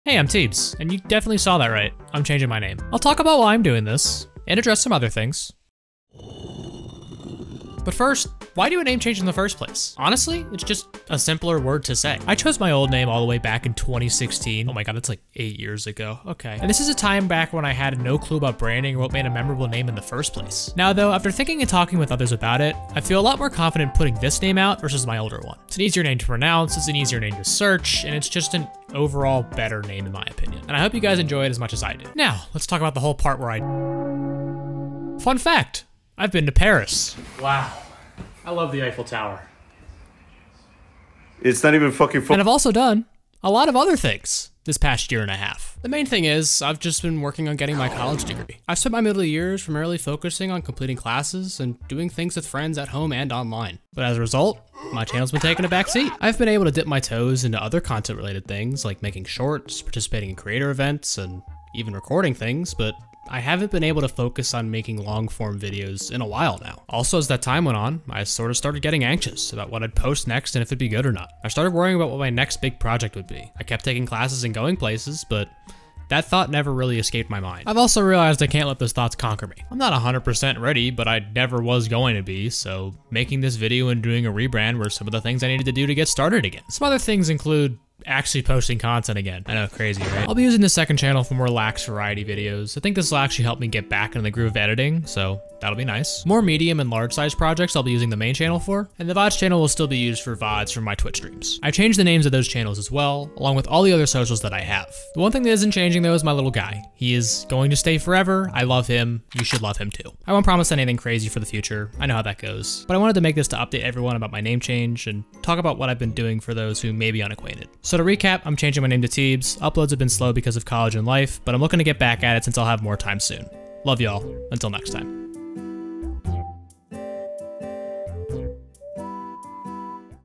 Hey, I'm Teebs!
I'm rebranding! (I'm not kidding)
I'll be switching to a new name ("Teebs") going forward! If you're interested in hearing about why and where I've been, stick around for 2 minutes while I explain things.

✨ALL SOCIALS HAVE SWITCHED TO TEEBS! ✨
Find all of them here!